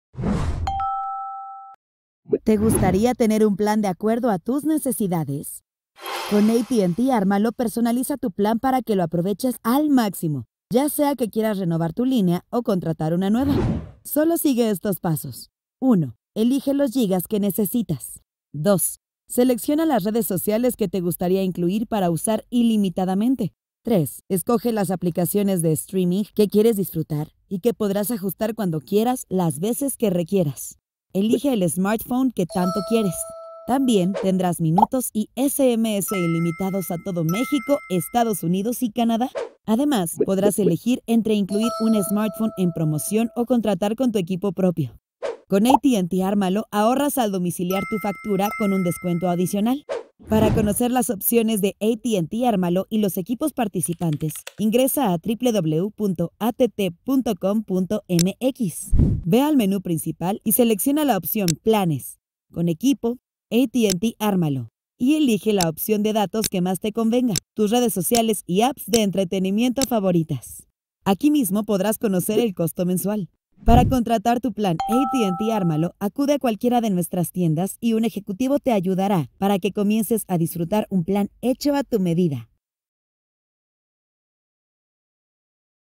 Conoce AT&T Ármalo.
Con AT&T Ármalo, puedes personalizar tu plan de acuerdo a tus necesidades. Aquí te decimos cómo aprovecharlo al máximo.